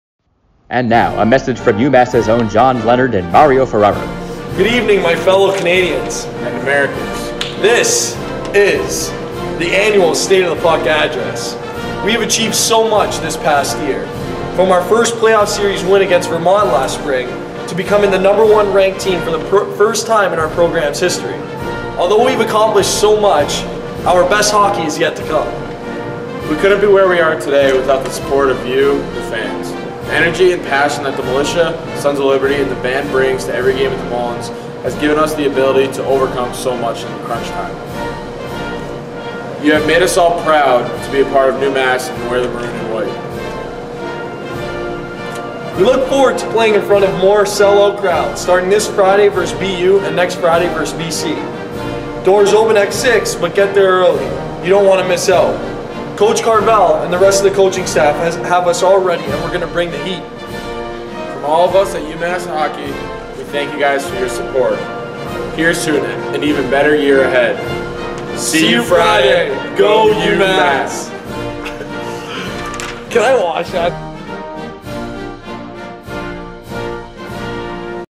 State of The Puck Address 2019
In honor of the State of the Union Address, we present to you the...

"State of the Puck Address"

Featuring San Jose Sharks draftees John Leonard and Mario Ferraro from our own UMass Hockey.

Producer/Director: Michael Bergman